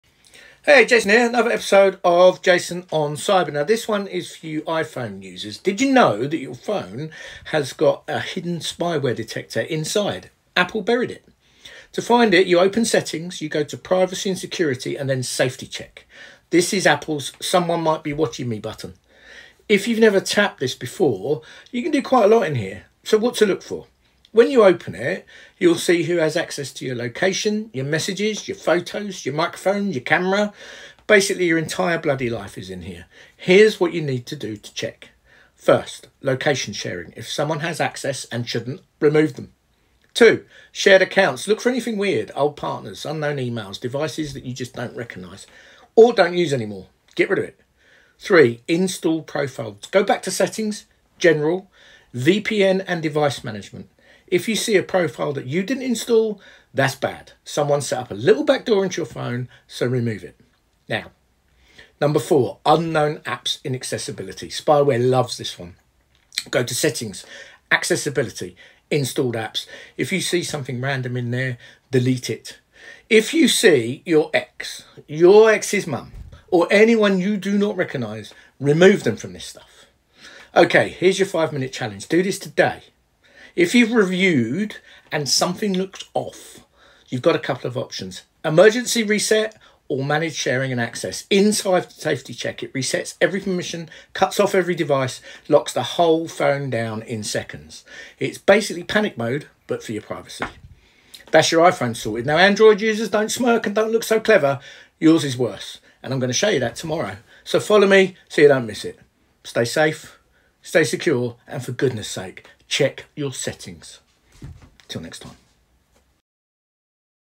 Your iPhone Has a Hidden Spyware Menu — Check It Before Bed.
Most iPhone users have never seen this setting before — but you need to check it today.

Quick walkthrough, zero tech jargon.